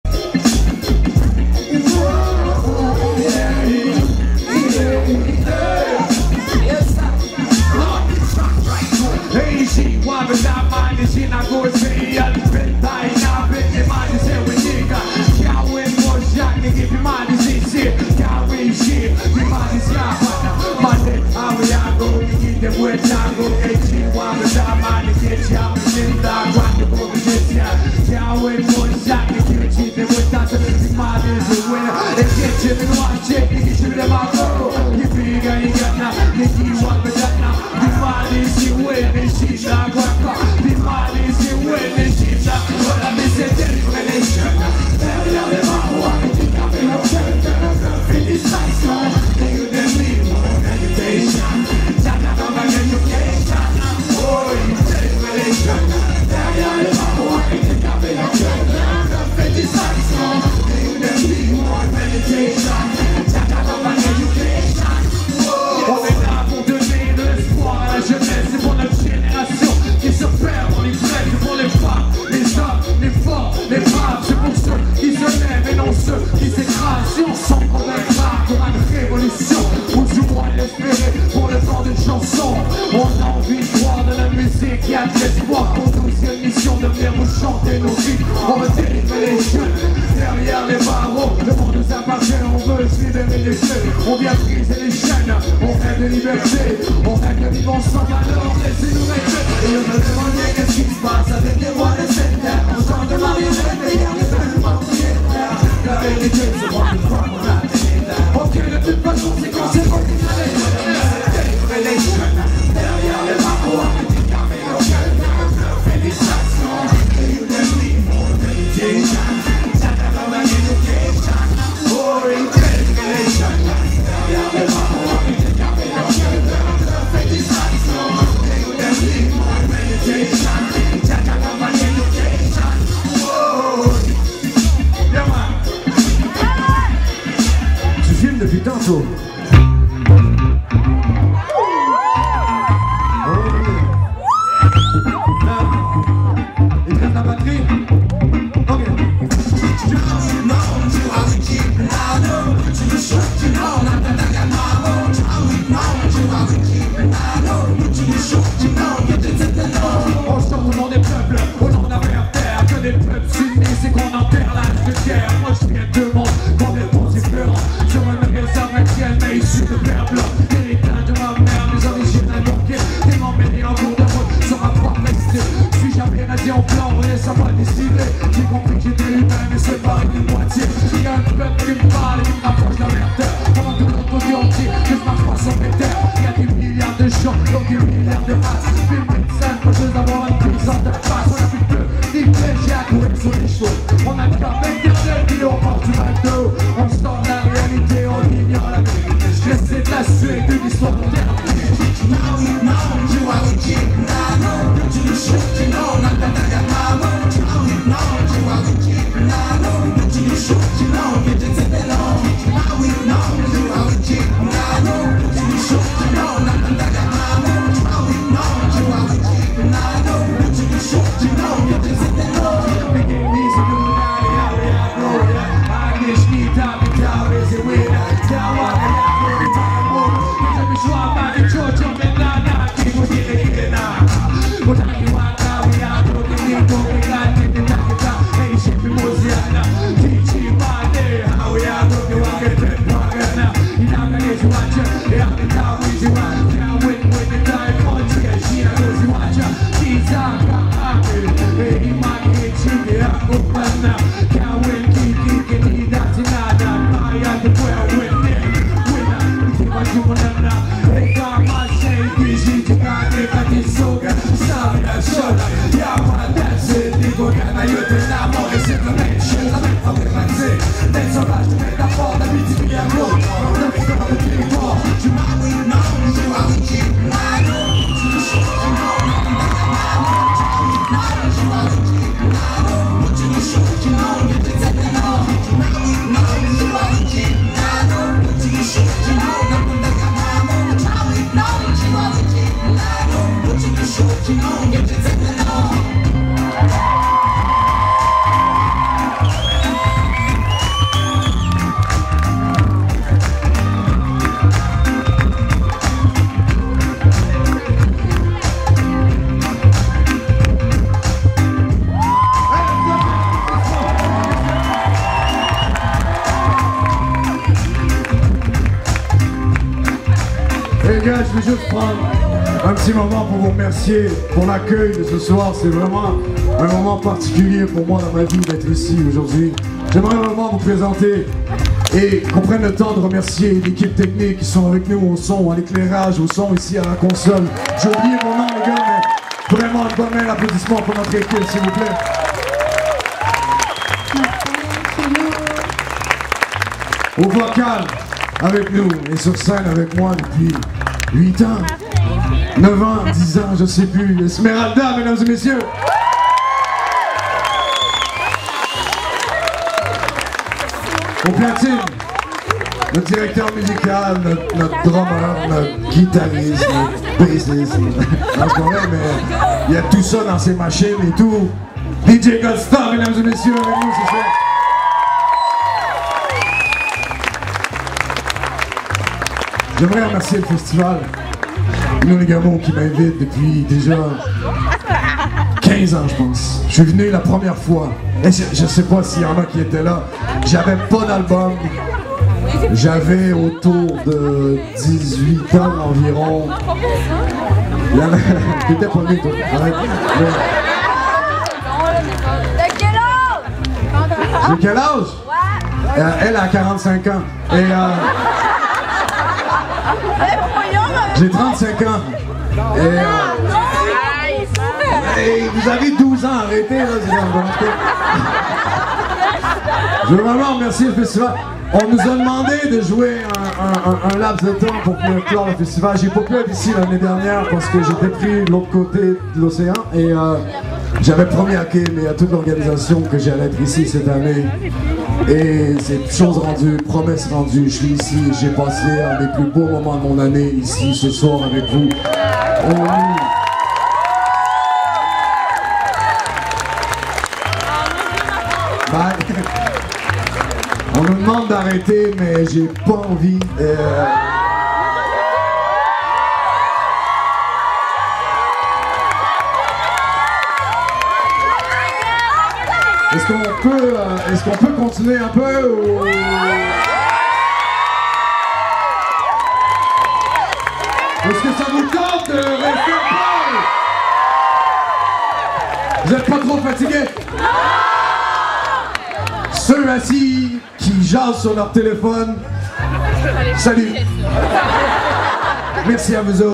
Samian, Innu Nikamu 2018 [11/13]
Samian, Innu Nikamu 2018, le 5 août 2018, Mani-utenam, QC. [11/13]